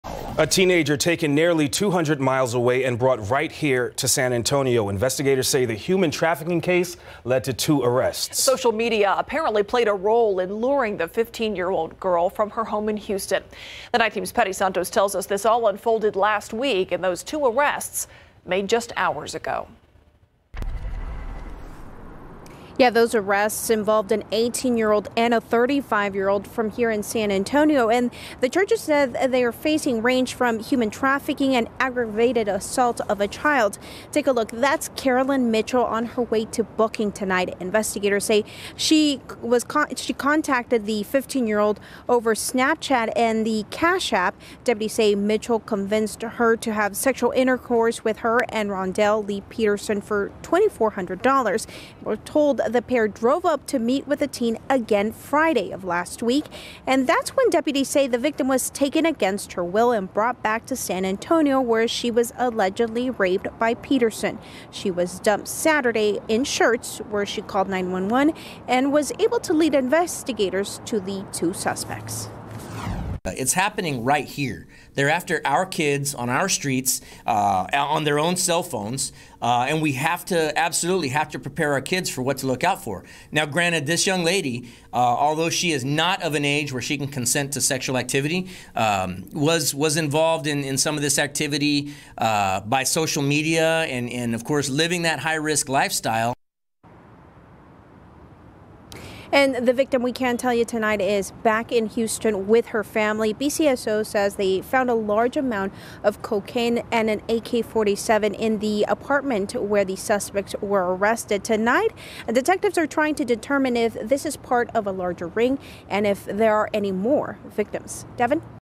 Two arrested in human trafficking case involving 15-year-old victim from Houston: BCSO
Two people were arrested on human trafficking charges after a teen made an outcry to the Bexar County Sheriff's Office, Sheriff Javier Salazar said.